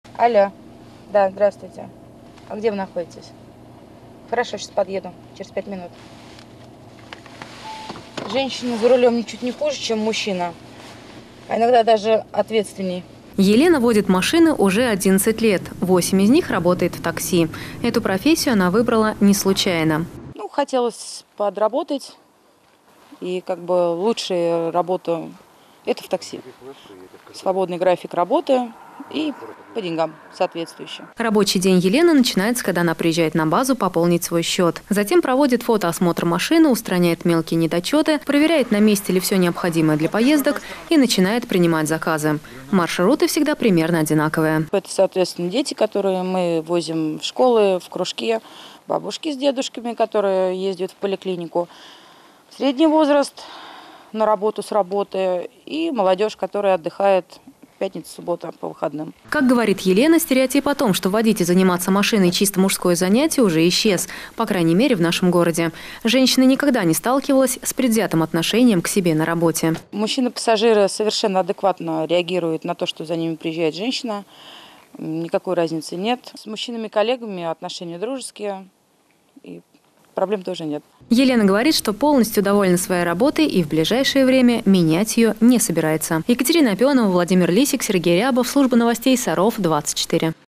Международный день таксиста
Они помогут успеть на деловую встречу, свидание, на вокзал или в аэропорт. 28 марта отмечается Международный день таксиста. Эту сложную, но интересную профессию выбирают и представительницы прекрасного пола.